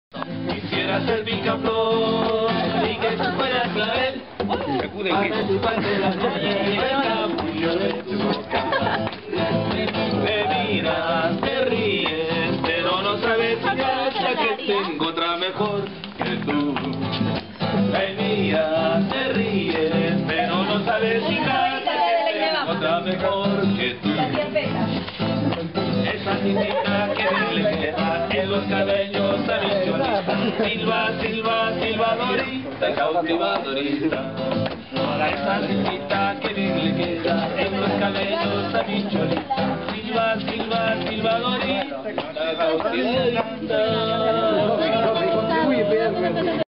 singing 3: tío emmanuel
tío emanuel cantando y otros tíos bailando, espectaculo improvisado para los sobrinos y el resto de la familia.